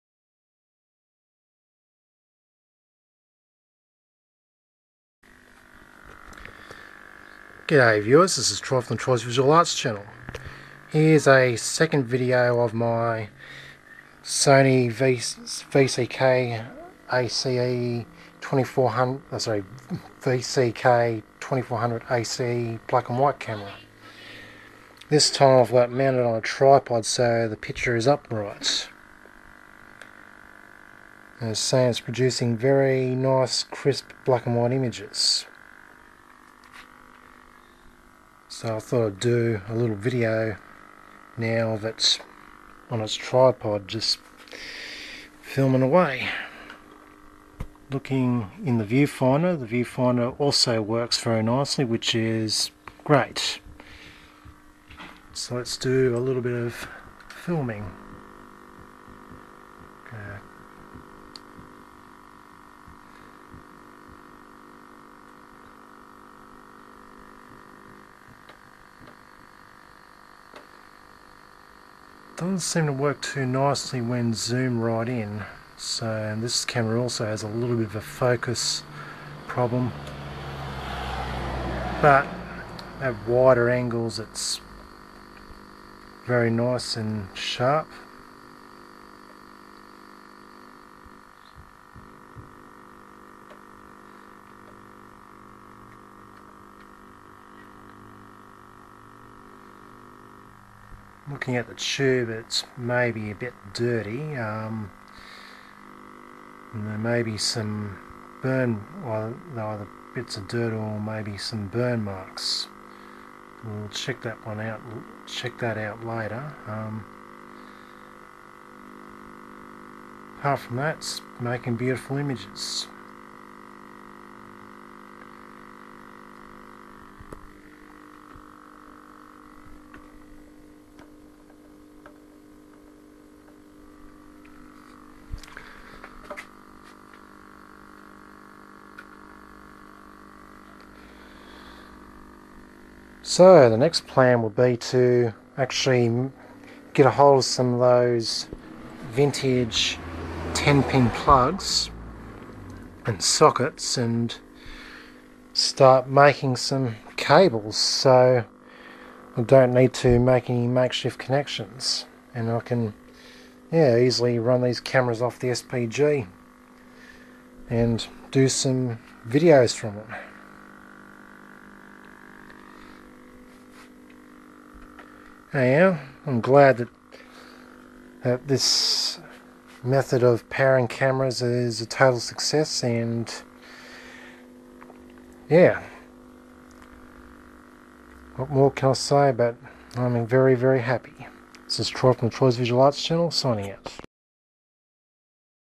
1967 Sony VCK-2400ACE B&W camera powered from SPG & 12V power supply (Part 2)
This is a video demo on operating a 1967 Sony VCK-2400ACE (European version of DVK-2400) B&W 2/3" vidicon tube B&W camera without its required portable DV-2400 1/2" reel-reel videotape recorder by means of a sync pulse generator (SPG) and a 12V power supply all connected to the camera's socket pins. I was inspired to try this experiment out after watching Richard Diehl's video "The Eight Dollar Camera" which he used a 12V battery as power source and his homemade SPG to operate his late 60s Sony DVK-2400 vidicon camera. So using the same techniques from his video I used a UTEL 2004 SPG to provide the horizontal and vertical syncs to the camera and I used a 13.8V power supply modified to output 12V as my 12V power source and I used the video output pins from the camera's socket to output video to the TV screen. After rigging it all up and firing it up, low and behold, it works!!!:D The camera makes pristine beautiful quality sharp B&W picture, the back focus yet needs adjustment as the picture blurs when zooming in on long distance shots, the tube and lens was initially dirty but have just cleaned it so it's now it makes absolutely perfect B&W pictures which I consider to be good as a broadcast grade B&W vidicon camera!:D Anyhow these portable cameras require their appropriate portable VTRs to operate them because the VTR provides the horizontal and vertical sync pulses and 12V to the camera for it to operate. With this method you can run the camera without the need of its VTR. Anyhow I am very happy this camera works great!!!:D